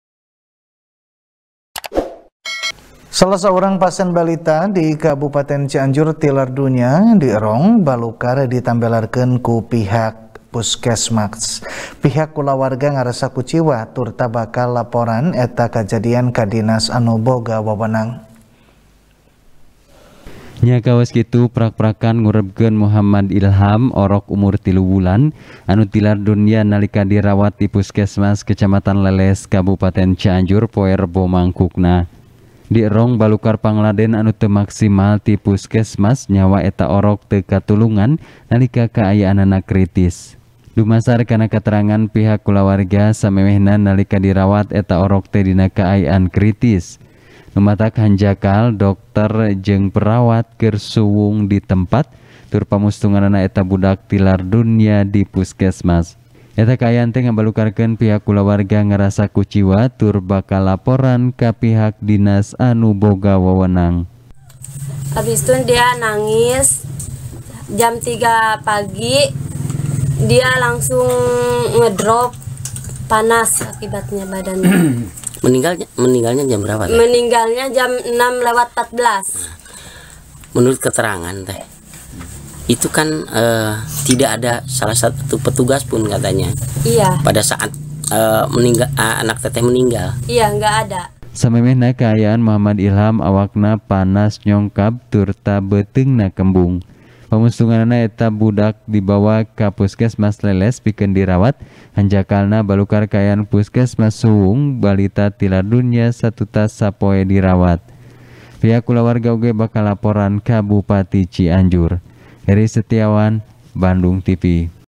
Pasén Balita Tepi Ka Tilar Dunya, Dierong Ditambélarkeun Puskesmas| TP | BANDUNGTV
TP | BANDUNGTV | DERI FIRMAN | Salah Saurang Pasen Balita Di Kabupaten Cianjur Tilar Dunya, Dierong Balukar Ditambelarkeun Ku Pihak Puskesmas. Pihak Kulawarga Ngarasa Kuciwa Turta Bakal Laporan Eta Kajadian Ka Dinas Anu Boga Wewenang.